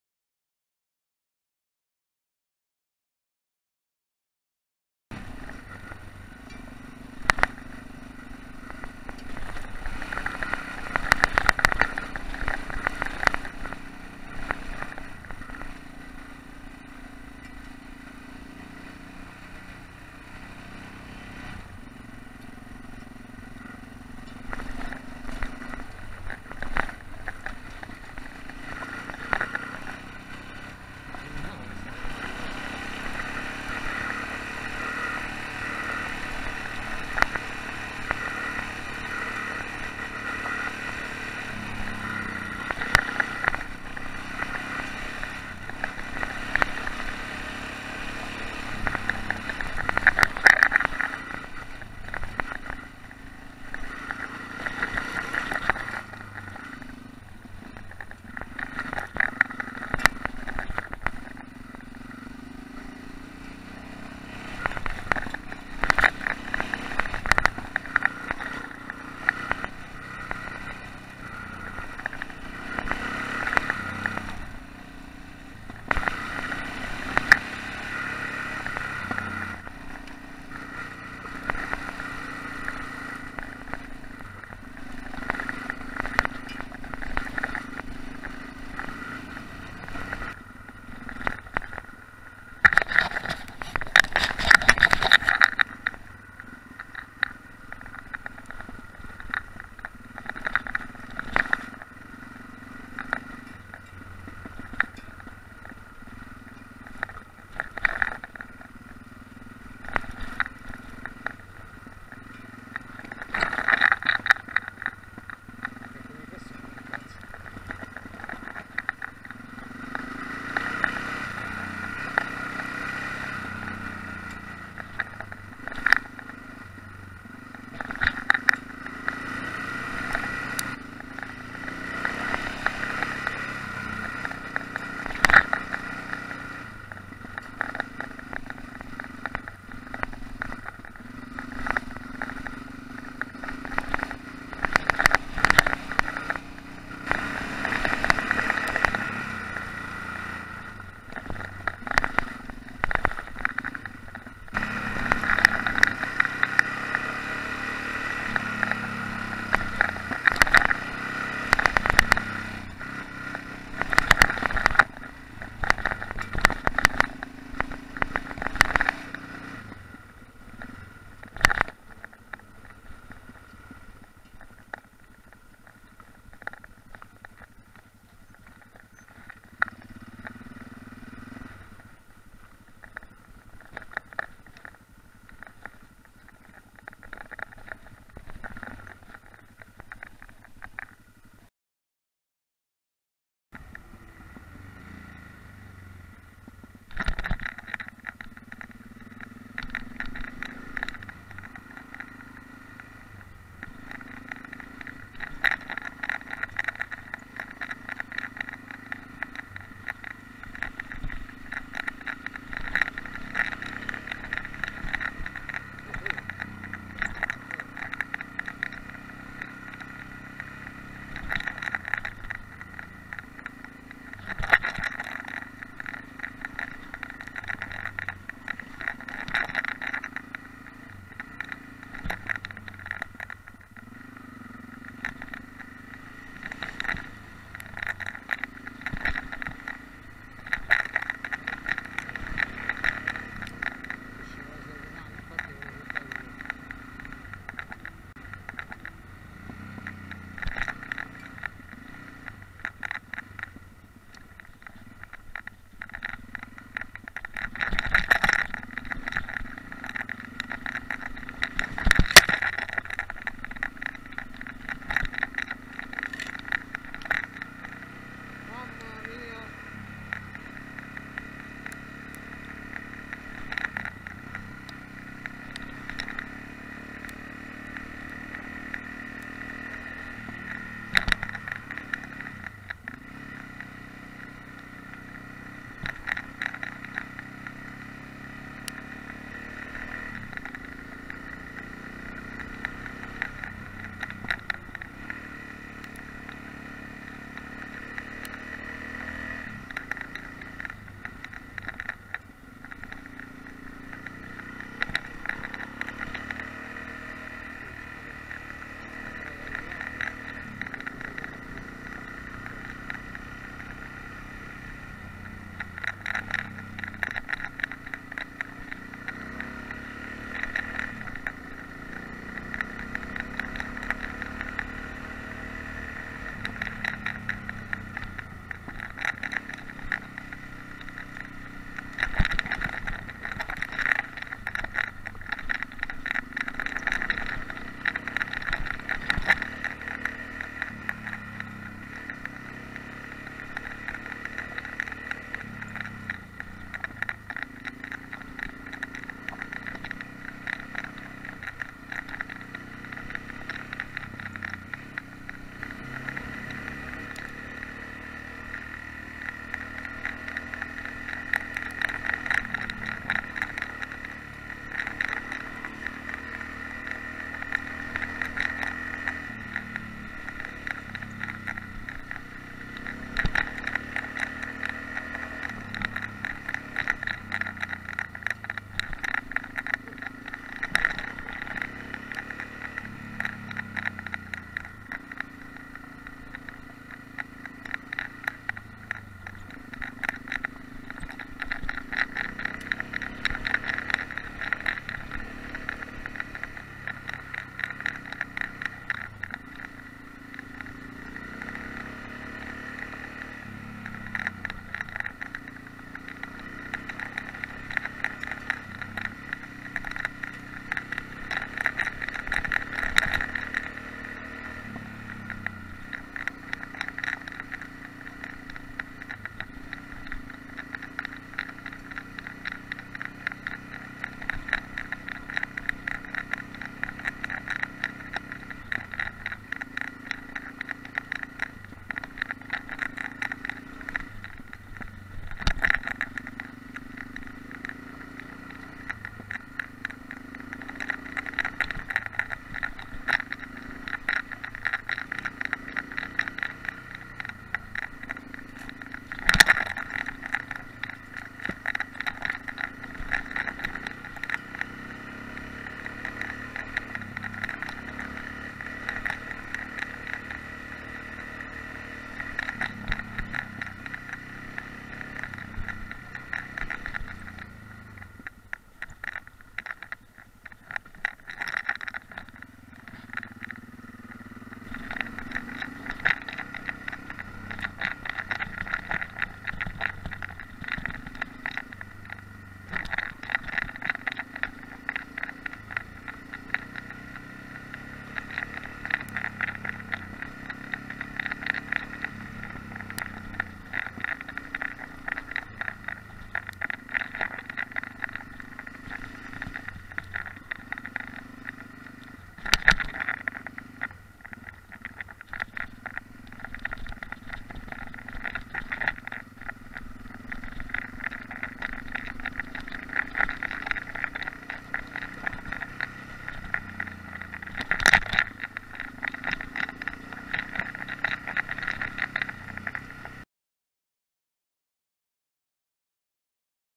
XT660X sterro enduro Farnesiana Allumiere del 2/01/2015
Uscita in off con la mia xtx